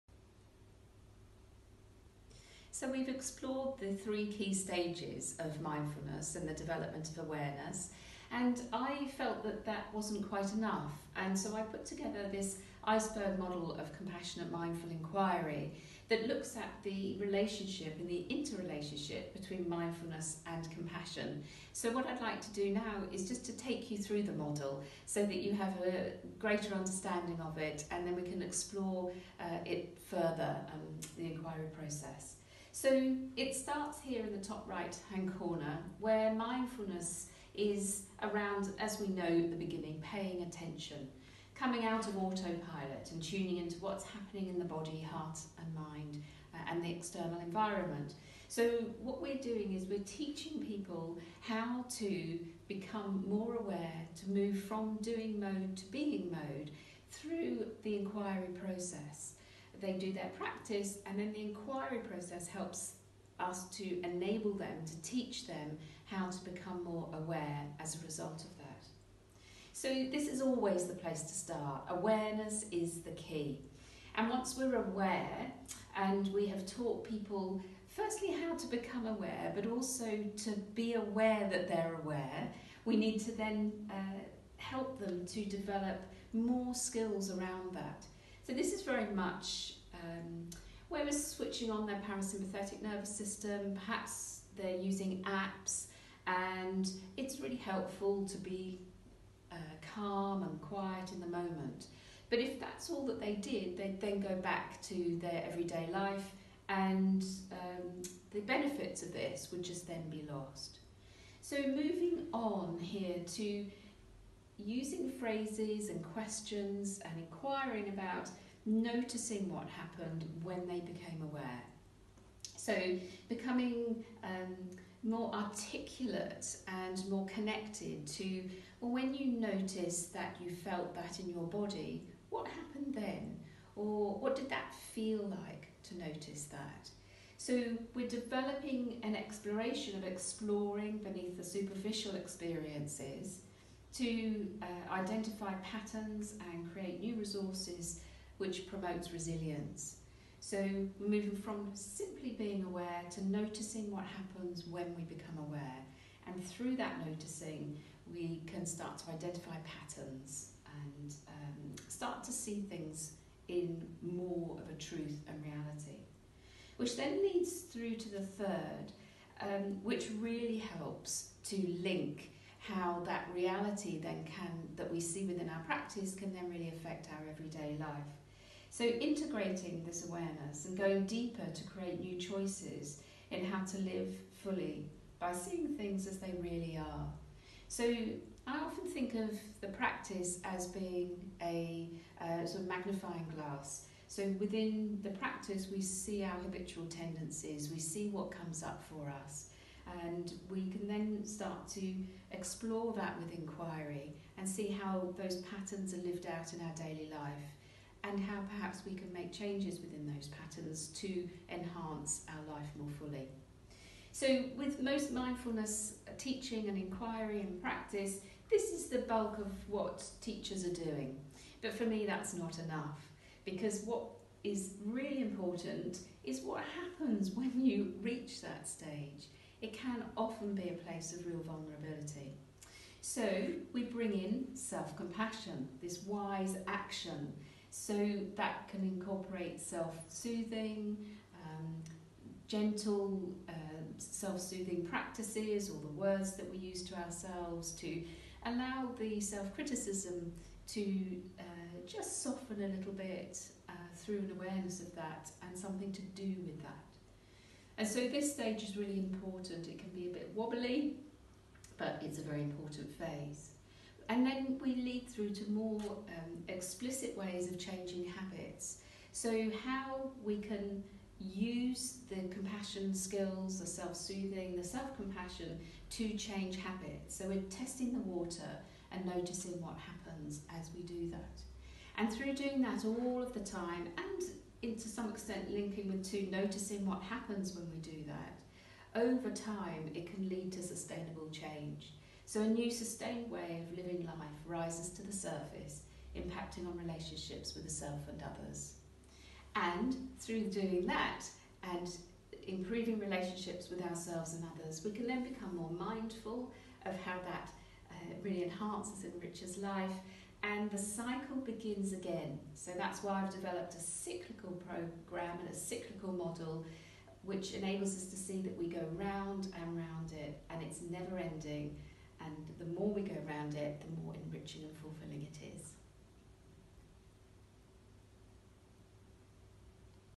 Explaining the Compassionate Mindful Inquiry model
Co-founder of MindfulnessUk, Karen Atkinson, introduces her new model of inquiry, which explcicitly explores the role of compassion